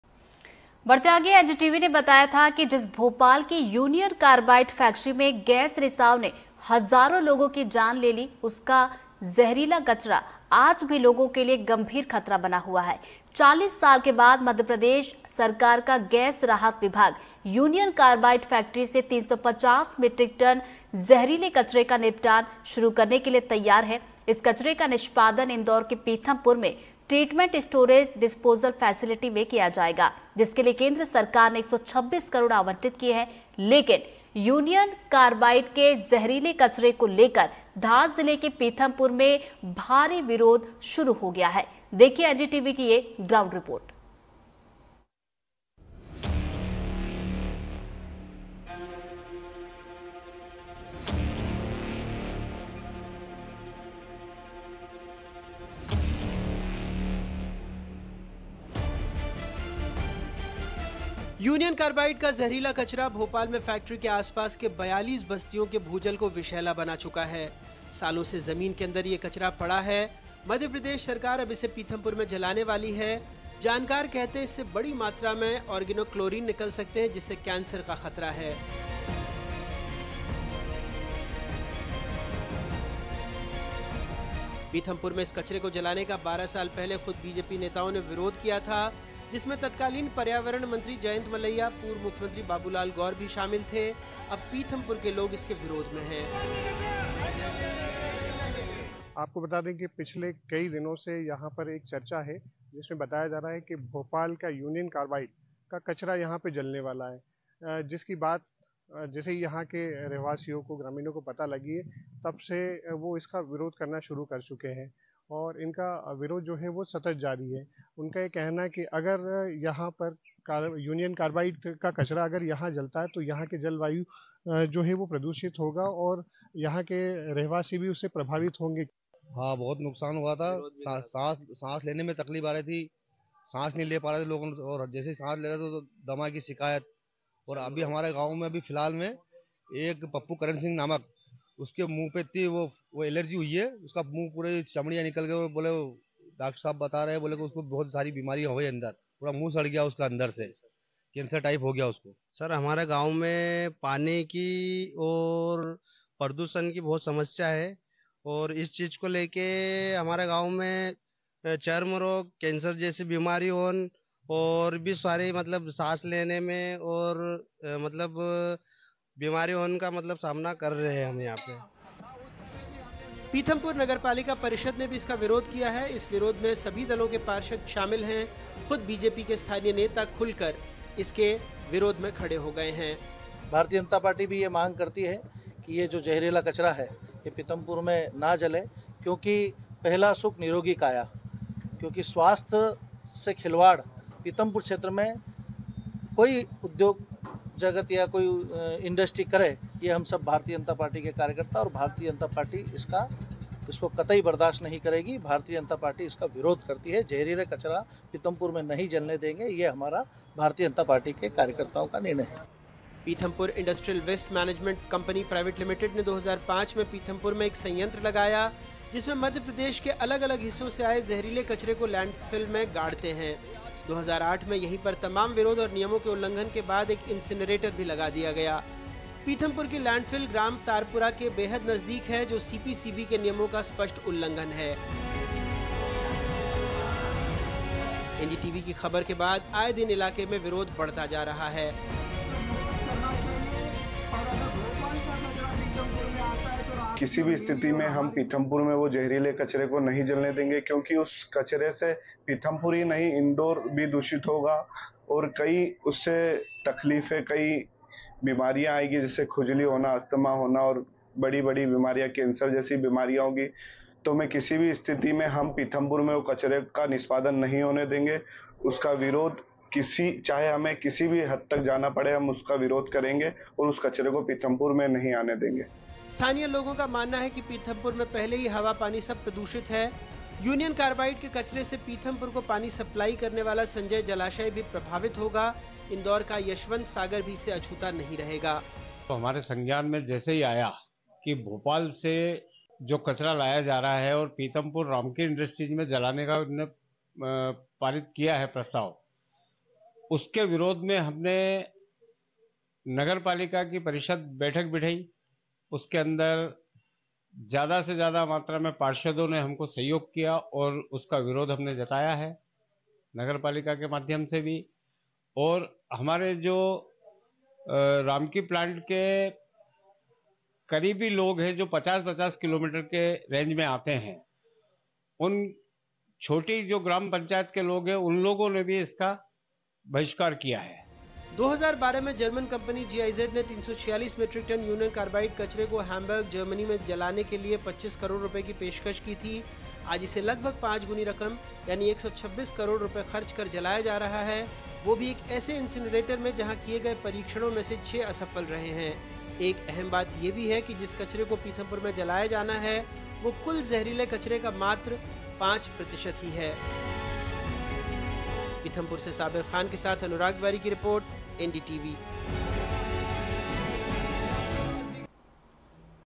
Bhopal Gas Tragedy: MP सरकार कचरा जलाने के लिए तैयार फिर लोग क्यों कर रहें विरोध क्यों?। CM Mohan
Bhopal Gas Tragedy: घटना के 40 साल बाद मध्य प्रदेश (MP) सरकार का गैस राहत विभाग यूनियन कार्बाइड फैक्ट्री (Union Carbide Factory) से 350 मैट्रिक टन ज़हरीले कचरे का निपटान शुरू करने के लिए तैयार है. लेकिन, यूनियन कार्बाइड के ज़हरीले कचरे (Poisonous Waste) को लेकर धार जिले के पीथमपुर में भारी विरोध शुरू हुआ गया है.